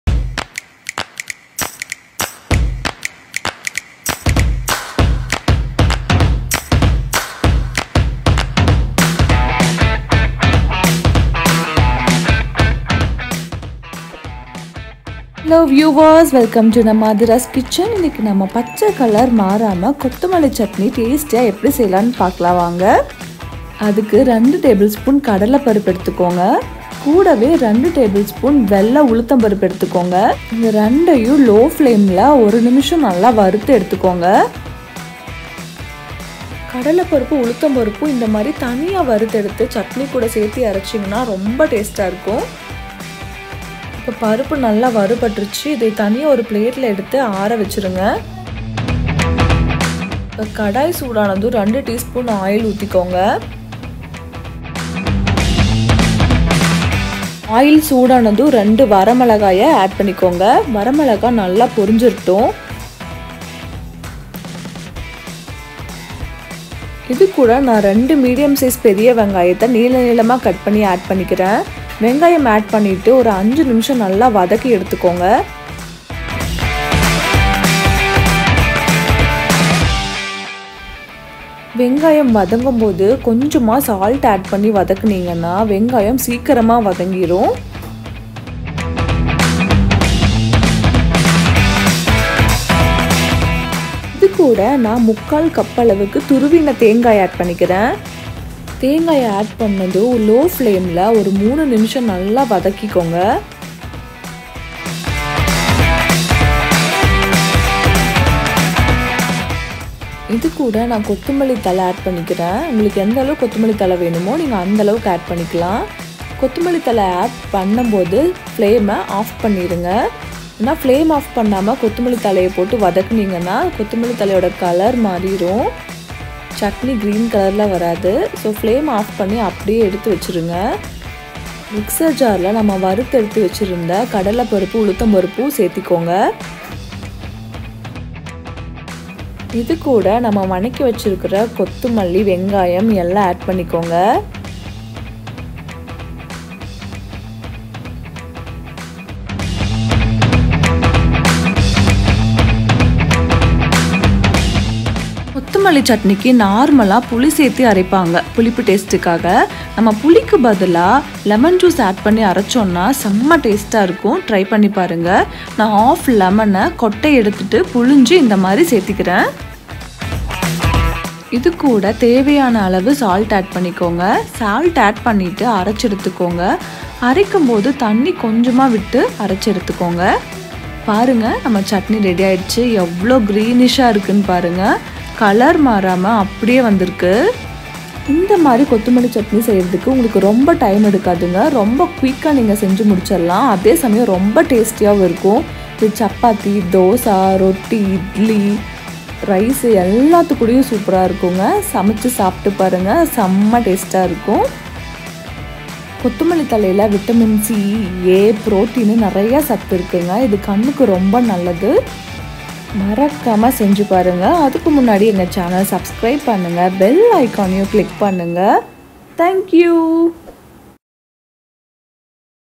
Green Corriander Chutney | கொத்தமல்லி சட்னி பச்சை நிறம் மாறாமல் செய்வது எப்படி
Aathiras Kitchen Shows you how to make green corriander Chutney without change in its colour,it's very healthy to eat with Dosa ,idly and Chappathi, it has lot of vitamin , so try it out